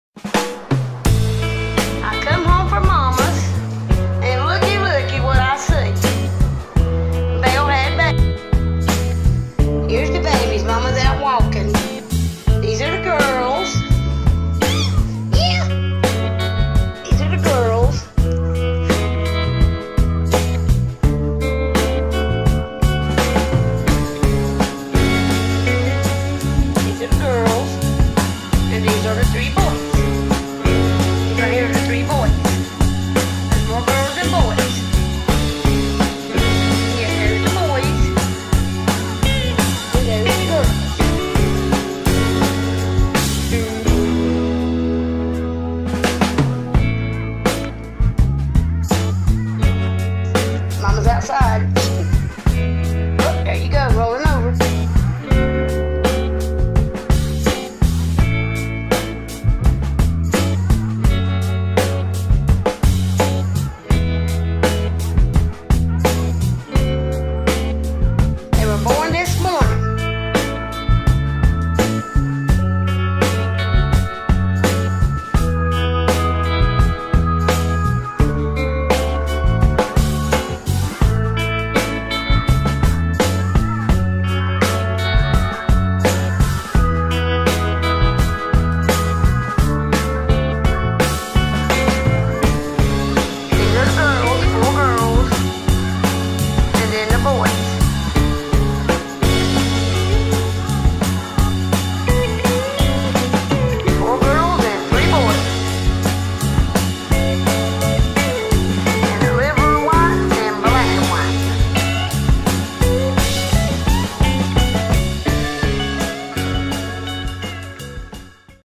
Bell with her puppys Englishspringerspaniel
Bell with her puppys that where just born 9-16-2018